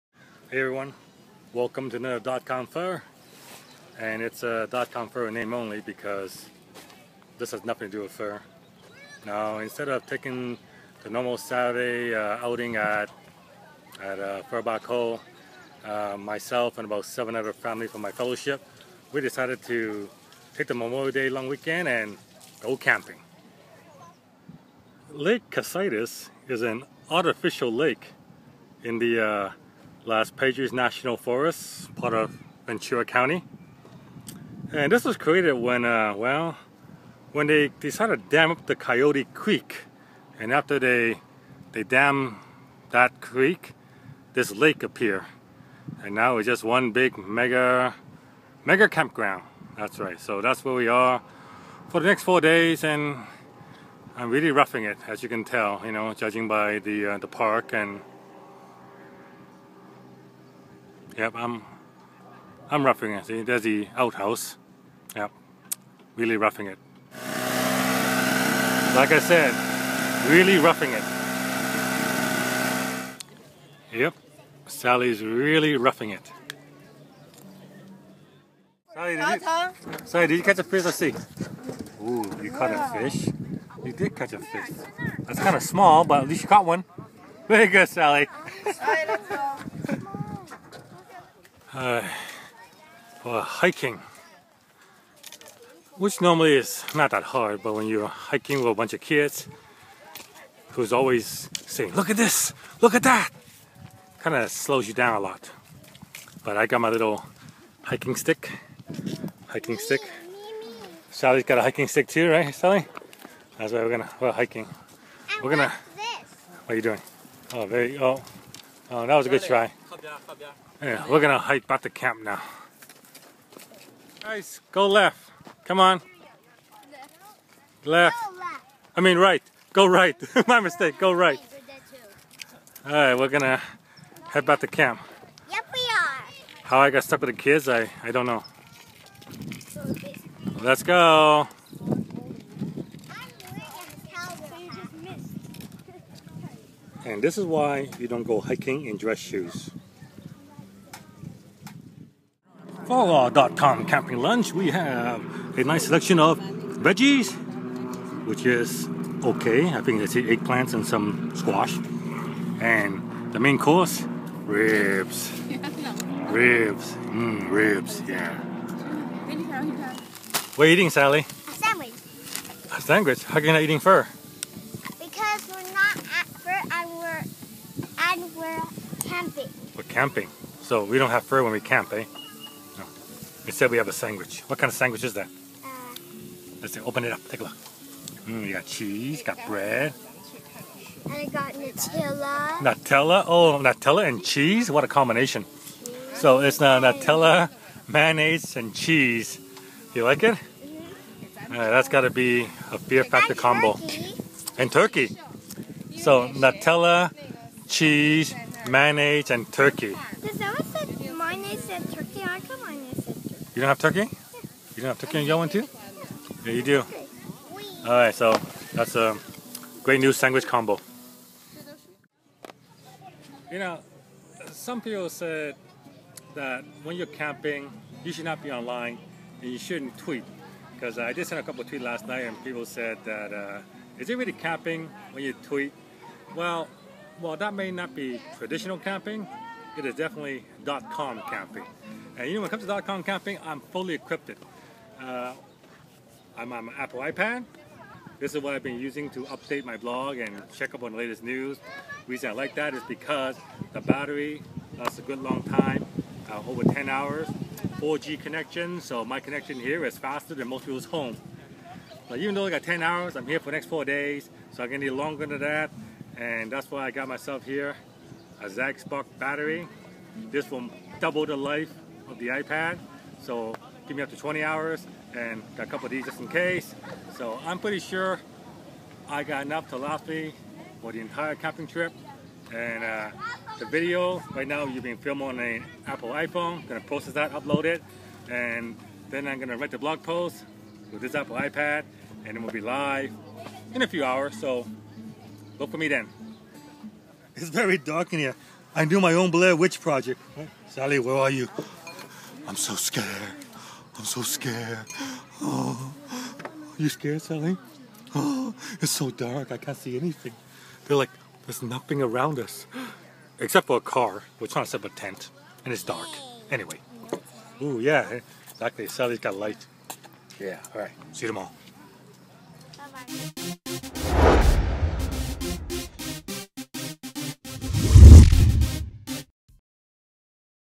Dot Com Pho - Dot Com Camping Edition
Instead of doing the normal Pho this week, we decided to do some Dot Com Camping! We were really roughing it!